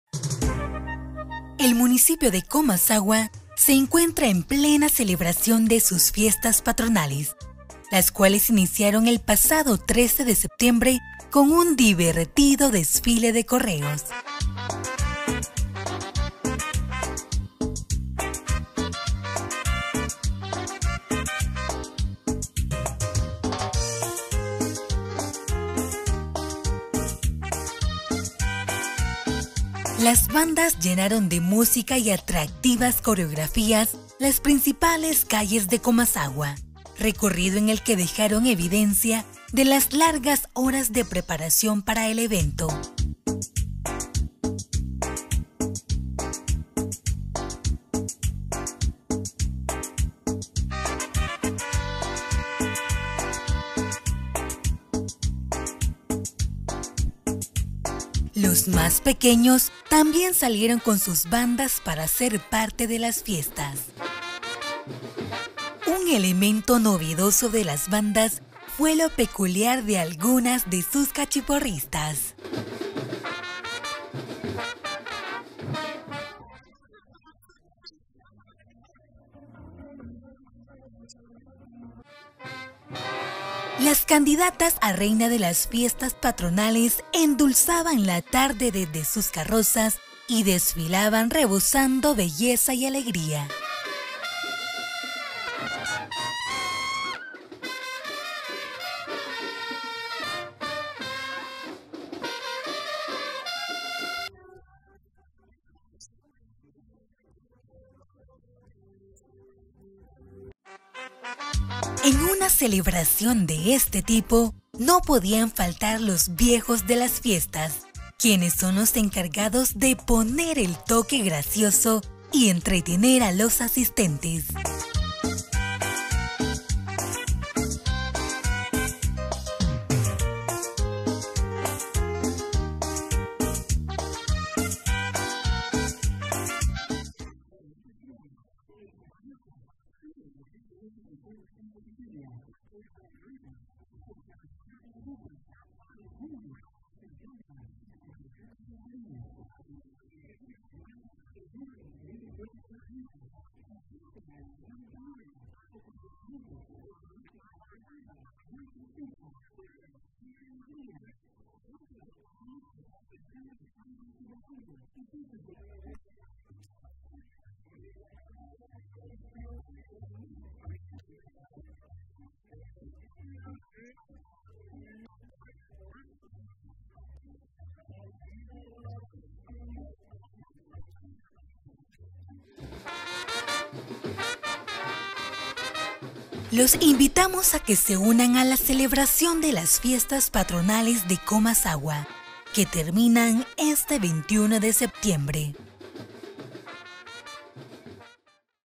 Estampas Salvadoreñas. Desfile de Correos en Comasagua, La Libertad, El Salvador
Comasagua es un bello municipio del departamento de La Libertad, rodeado de montañas y que todos los años se llena de colorismo en el marco de sus fiestas patronales en el mes de septiembre